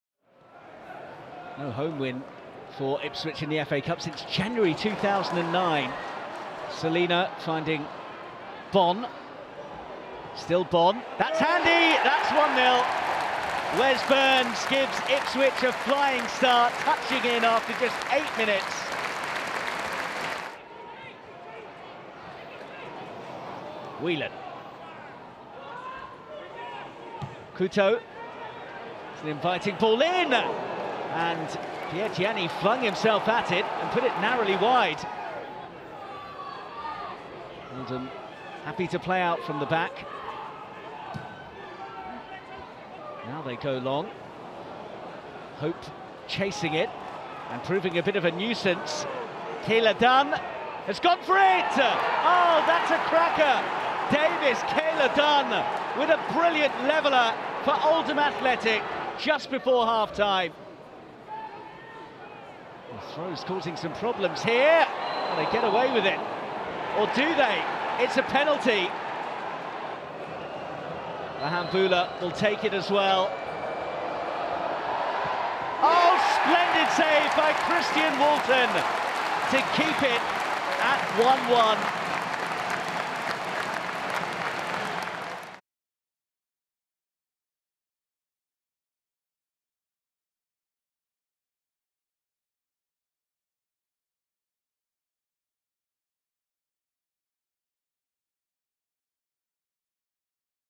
Ipswich Held by Latics | Ipswich Town 1-1 Oldham Athletic | Emirates FA Cup 2021-22
Ipswich Town and Oldham Athletic will go to a replay after neither side could force a winner in a tough encounter where Ipswich's Christian Walton on loan from Brighton & Hove Albion performed a last-minute penalty save to keep his side in the tie.

To find out more about The Emirates FA Cup visit:
TheFA.com/EmiratesFACup